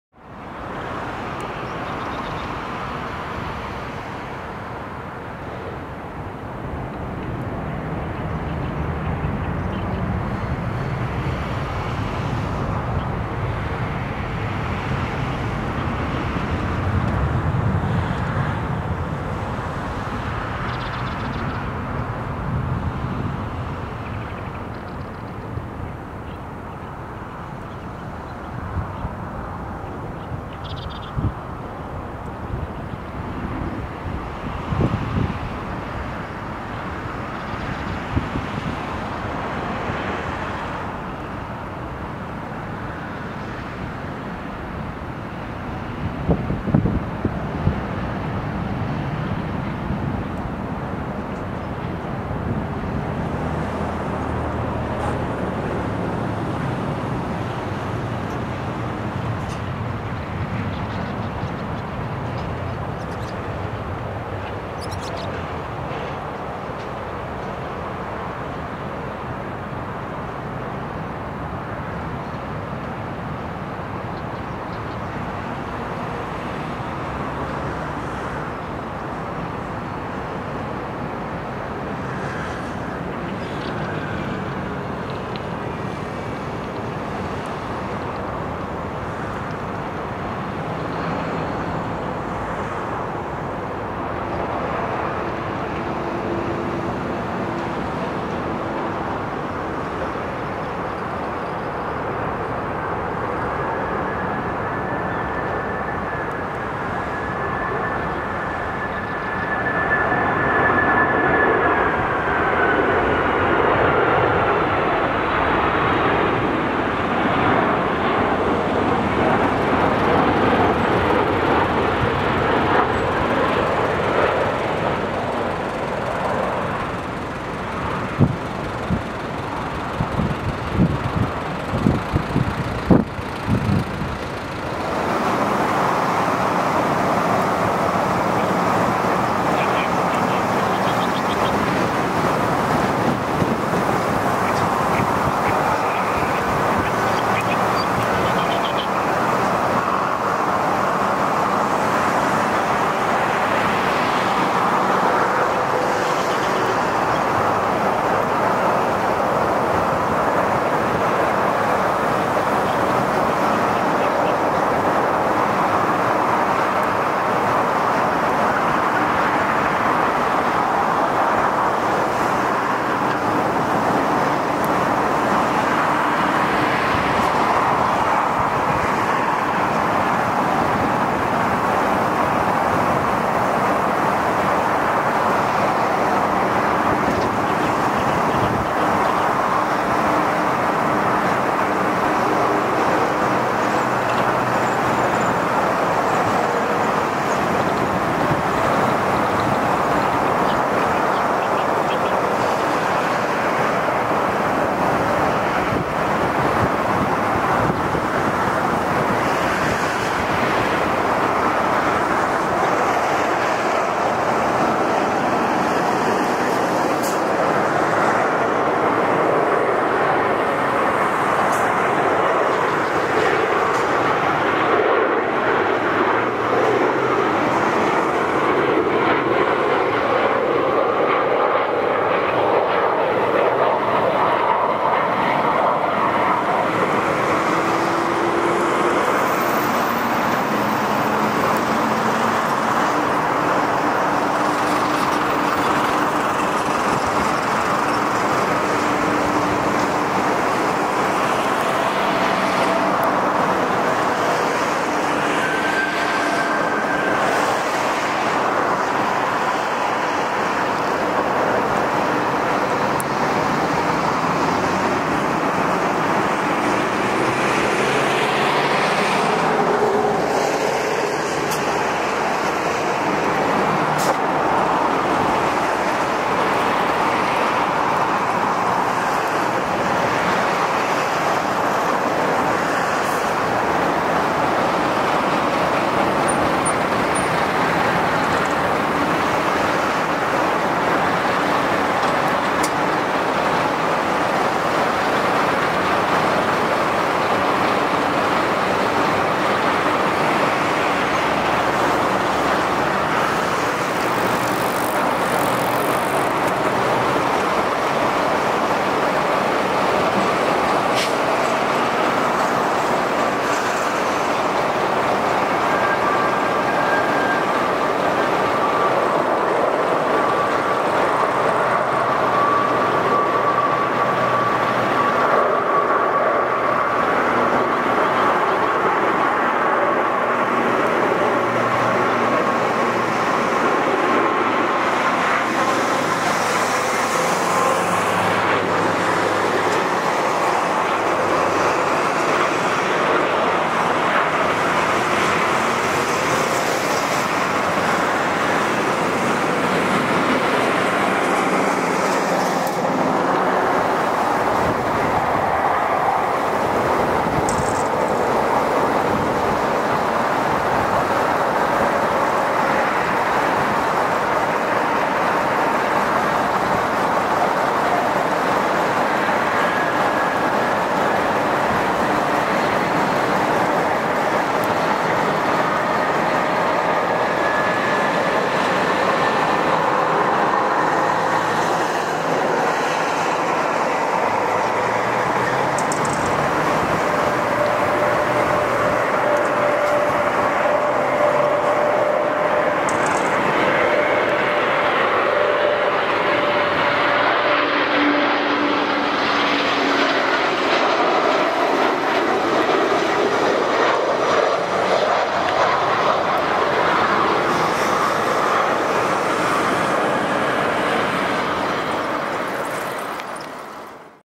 【伊丹空港】晴天での逆ランウェイ運用 RWY14L
まさかRWY14運用になっているとも思わず空港へ行ったら逆ランでラッキーでした(^^)　晴天での逆ランは本当に珍しい（風は当然ながら強風です）　今回はNew雲台のおかげで以前のように風でブレる事も少なく、動画においてもしっかりした雲台と脚は必要だと改めて思いました。

【Digital Camera】Canon EOS 7D MarkⅡ + EF-S55-250mm F4-5.6 IS STM
【Microphone】Panasonic RR-XS450
【Tripod+Head】Velbon Mark-7G + MVH502AH